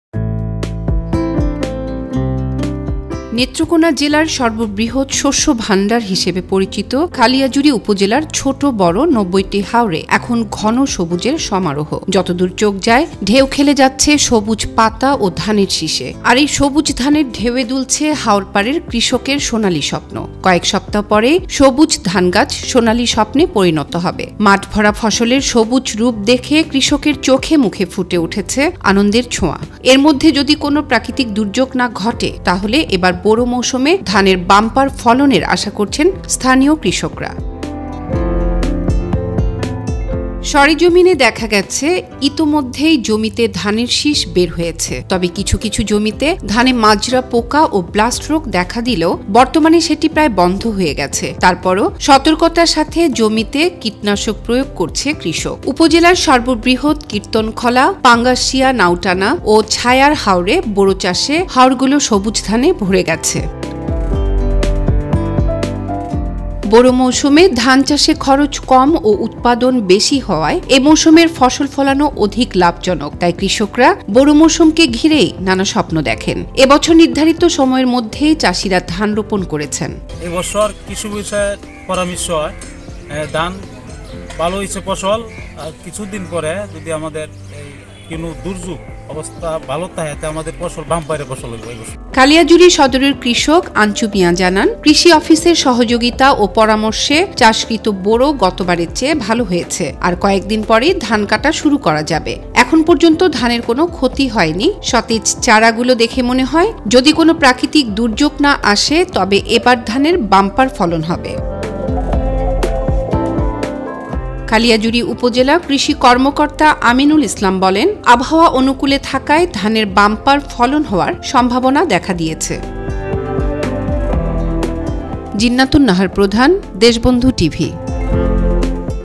খালিয়াজুরীর হাওরে বোরোর বাম্পার ফলনের আশা | দেশবন্ধু টিভি | DeshbandhuTV
নেত্রকোনা জেলার সর্ববৃহৎ শস্যভাণ্ডার হিসেবে পরিচিত খালিয়াজুরী উপজেলার ছোট বড় ৯০টি হাওরে এখন ঘন সবুজের সমারোহ। যতদূর চোখ যায় ঢেউ খেলে যাচ্ছে সবুজ পাতা ও ধানের শীষে। আর এই সবুজ ধানের ঢেউয়ে দুলছে হাওর পাড়ের কৃষকের সোনালি স্বপ্ন। কয়েক সপ্তাহ পরেই সবুজ ধানগাছ সোনালি স্বপ্নে পরিণত হবে। মাঠ ভরা ফসলের সবুজ রূপ দেখে কৃষকদের চোখে মুখে ফুটে উঠেছে আনন্দের ছোঁয়া। এর মধ্যে যদি কোনো প্রাকৃতিক দুর্যোগ না ঘটে তাহলে এবার বোরো মৌসুমে ধানের বাম্পার ফলনের আশা করছেন স্থানীয় কৃষকরা।

সরেজমিনে দেখা গেছে, ইতোমধ্যেই জমিতে ধানের শীষ বের হয়েছে। তবে কিছু কিছু জমিতে ধানে মাজরা পোকা ও ব্লাস্ট  রোগ দেখা দিলেও বর্তমানে সেটি প্রায় বন্ধ হয়ে গেছে। তারপরও সতর্কতার সাথে জমিতে কীটনাশক প্রয়োগ করছে কৃষক। উপজেলার সর্ববৃহৎ কির্তনখলা, পাঙ্গাসিয়া নাউটানা ও ছায়ার হাওরে বোরো চাষে হাওরগুলো সবুজ ধানে ভরে গেছে।

বোরো মৌসুমে ধান চাষে খরচ কম ও উৎপাদন বেশি হওয়ায় এই মৌসুমের ফসল ফলানো অধিক লাভজনক, তাই কৃষকরা বোরো মৌসুমকে ঘিরেই নানা স্বপ্ন দেখেন। এ বছর নির্ধারিত সময়ের মধ্যেই চাষিরা ধান রোপণ করেছেন।

"Deshbandhu TV" is the Bangladesh Government Approved Reliable  TV Channel